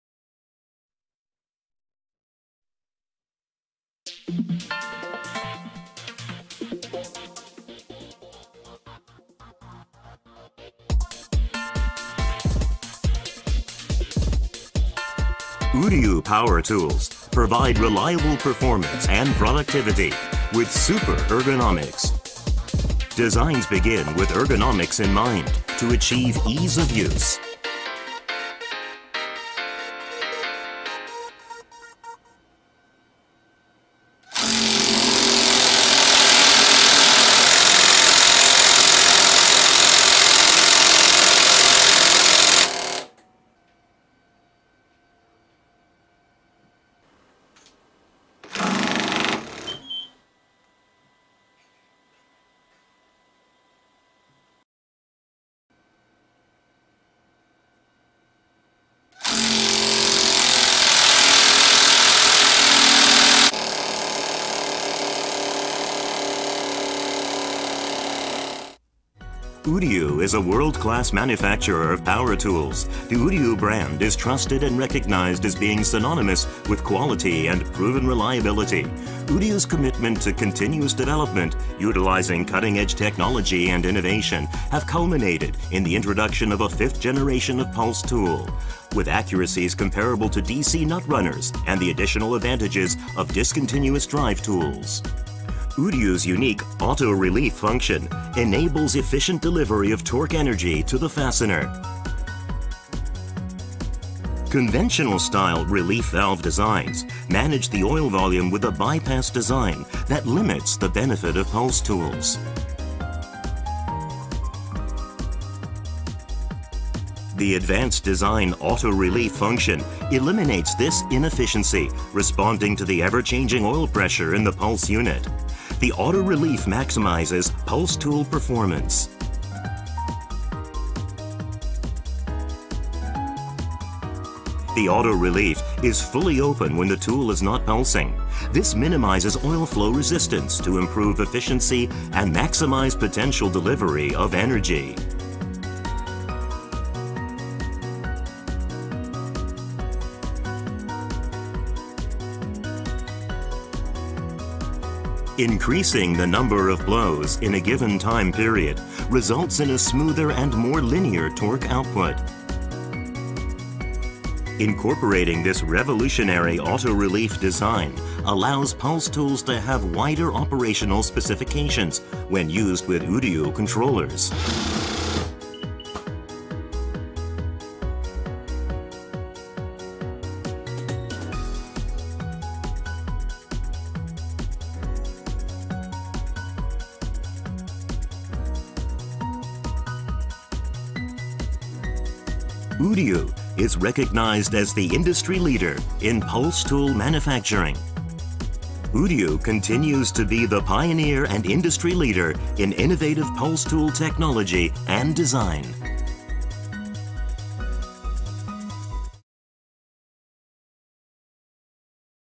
Uryu - Tecnológia Hidro pulsativa
Explicativo da fabricante Uryu sobre a tecnologia de funcionamento das apertadeiras Hidro pulsativas.